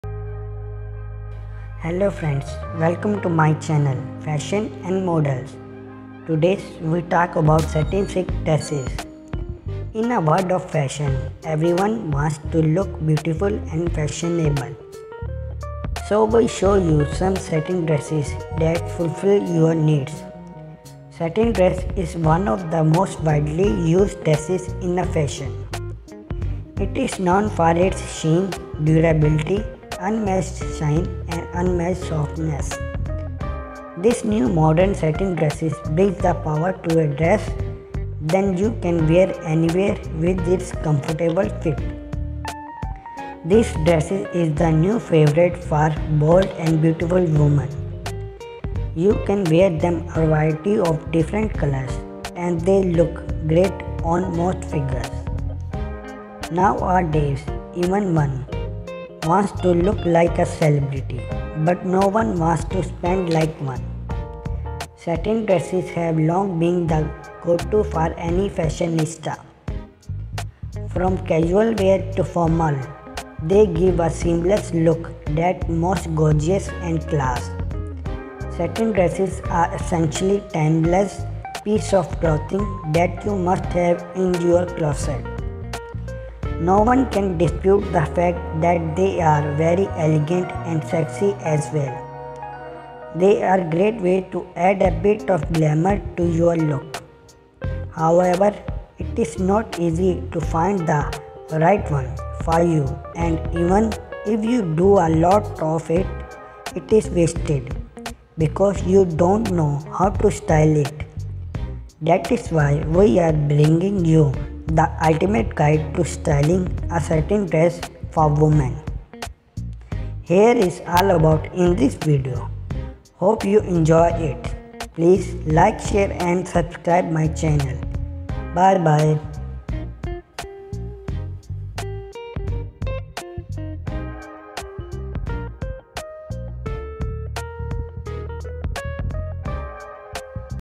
Satin Silk Dress Collection || Models In Satin silk Dress
Hello friends todays I in this video we are going to show you some styles and combination of satin silk dresses. women loves to wear dress. satin silk dresses are very popular in today's fashion world. you can wear it anytime anywhere. so watching the video and enjoy it

Note: all the information contained does not guarantee 100 % accuracy and all images that are used in this videos are from Instagram and other social media websites